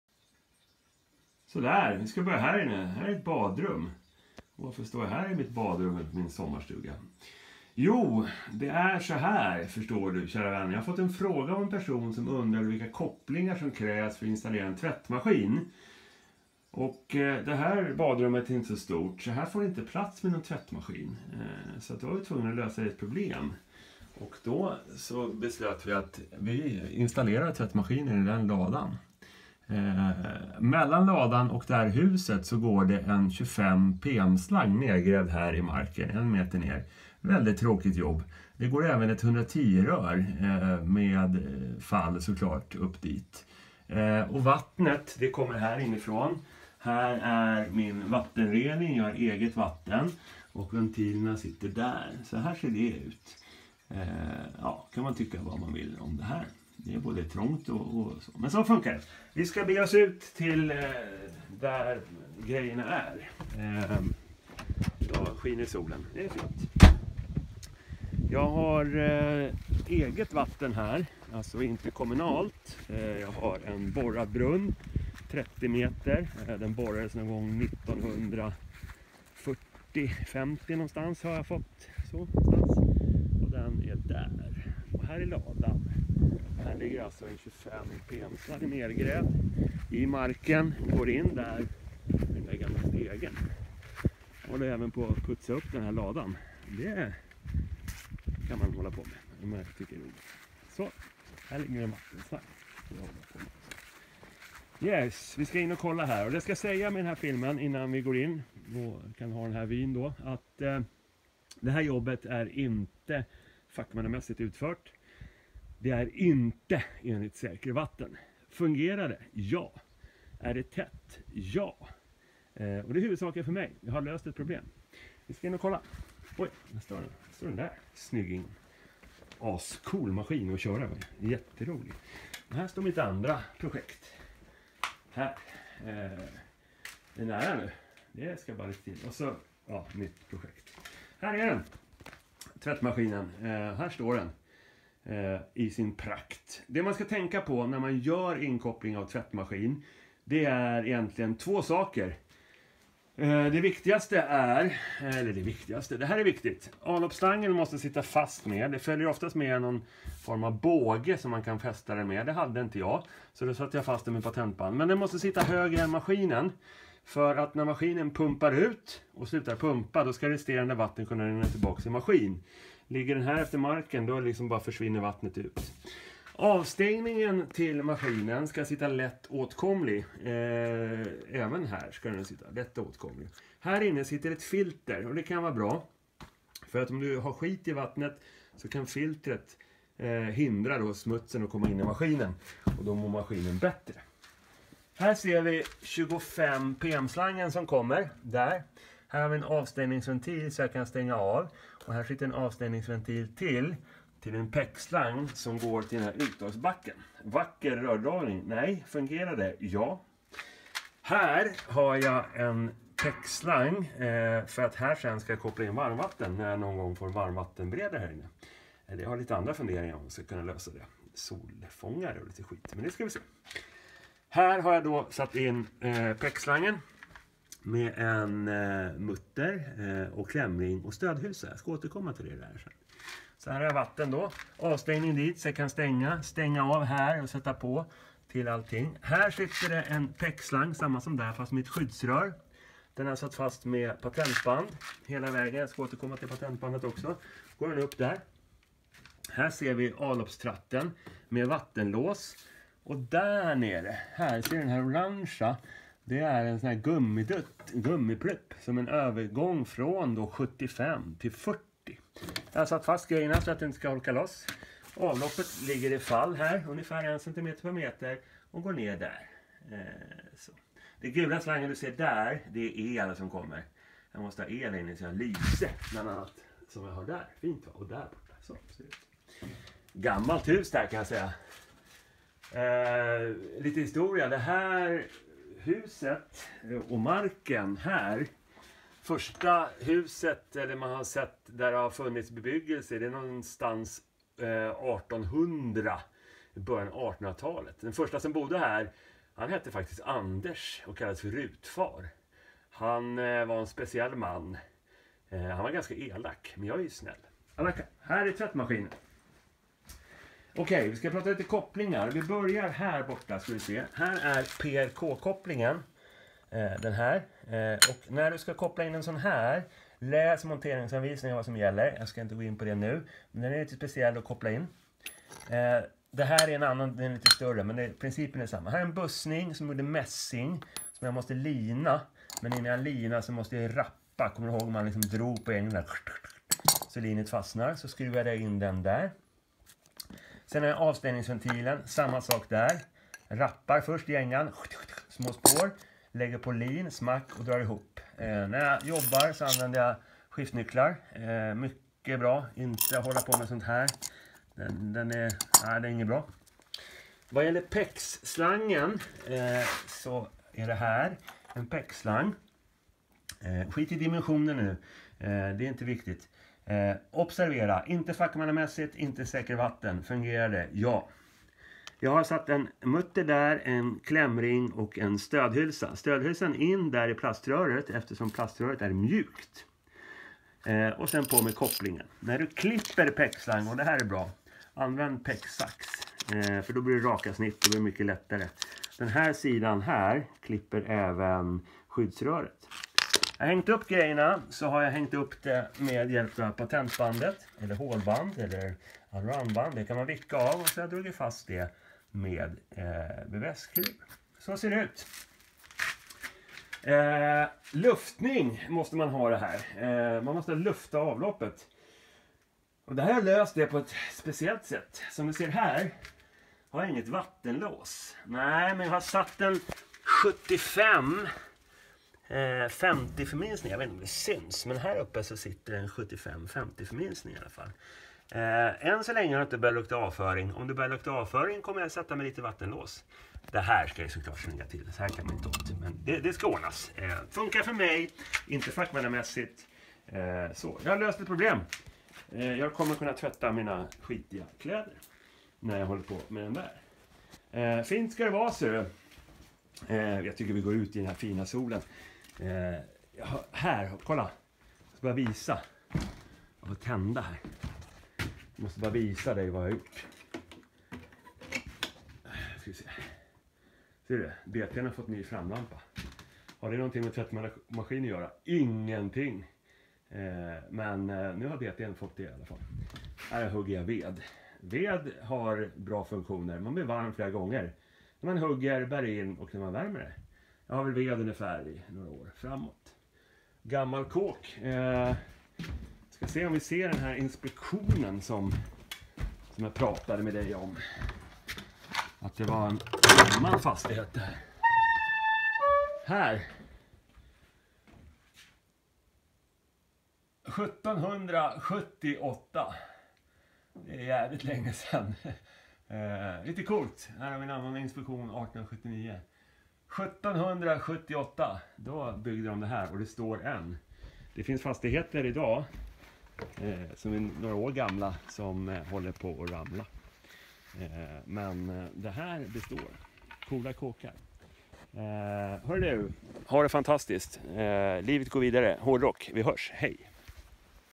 Inkoppling av tvättmaskin
Jag var tvungen att lösa ett problem och har gjort en inkoppling av en tvättmaskin. Jag vill vara tydlig med att det som visas i filmen INTE är fackmannamässigt utfört det är INTE heller gjort enligt säkervatten. Det fungerar för mig. Om du ska koppla in en maskin hos en kund så ska du göra enligt gällande branschkrav, detta för att försäkringar osv ska gälla. Som sagt jag har löst ett problem som fungerar för mig.

OBS!! jag har fått lite påpekande angående avloppslangen från maskinen. Slangens "böj" måste vara i jämnhöjd med maskinens överkant. Min slang sitter alltså lite väl högt, men det funkar!